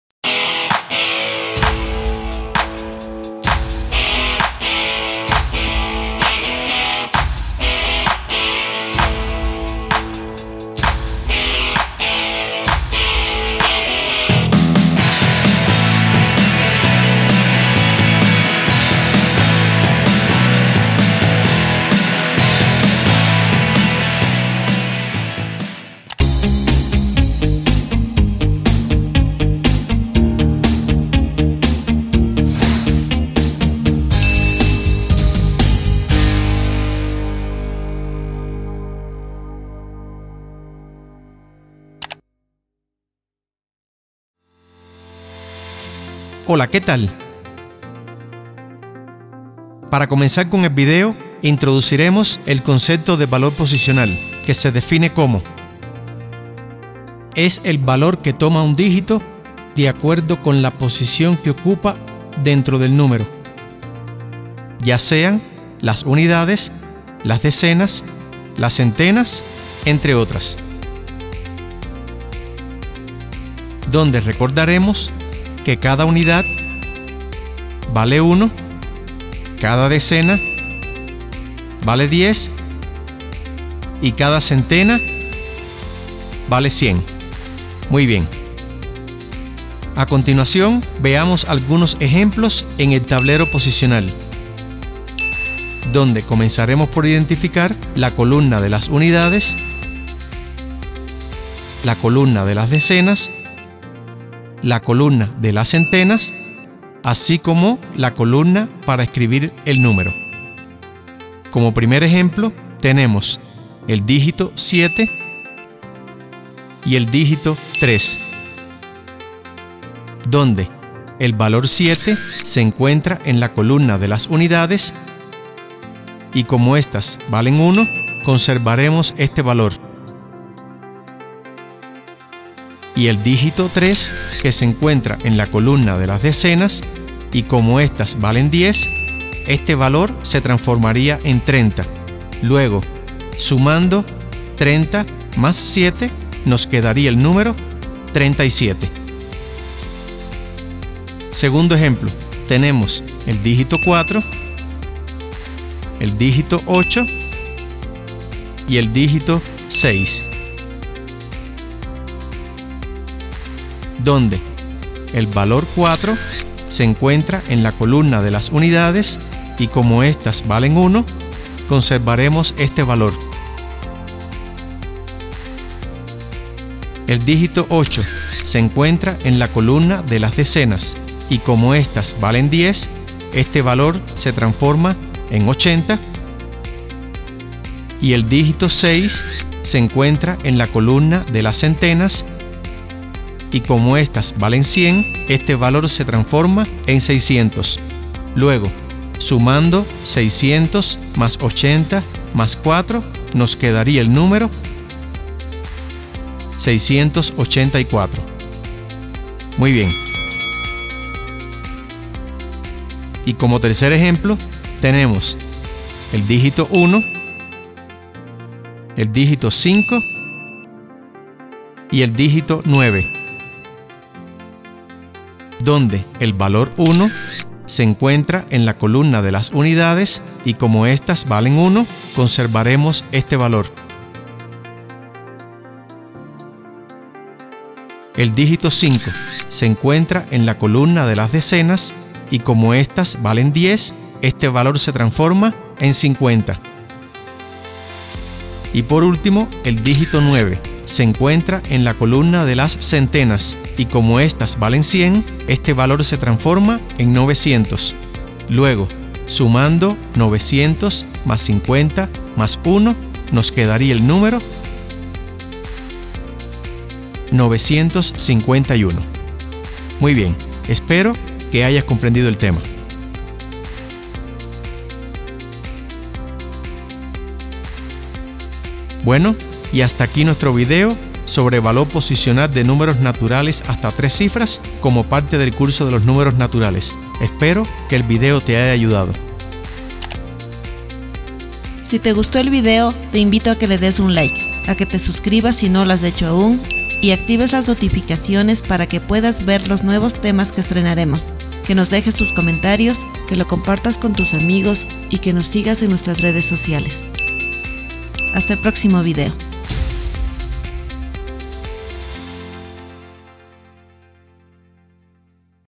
¡Aprende el Valor POSICIONAL de Números hasta 3 Cifras Paso a Paso! 🤯✨
🎬 ¡Hola, matemáticos en acción! 🔢🚀
Hoy vamos a descubrir un concepto clave para entender los números: ¡el valor posicional en números naturales de hasta 3 cifras! 🤯✨
📌 ¿Por qué un mismo número puede valer diferente dependiendo de su posición? 🤔
📌 ¿Cómo identificar fácilmente unidades, decenas y centenas? 🔍
📌 ¿Y qué truco puedes usar para descomponer números sin complicaciones? 😏
En este video, te explicaré de manera fácil, visual y con ejemplos claros cómo funciona el valor posicional, para que entiendas los números como un verdadero experto. 😎✅
Así que toma tu cuaderno, afila tu mente y prepárate, porque esto empieza… ¡YA! 🚀🔥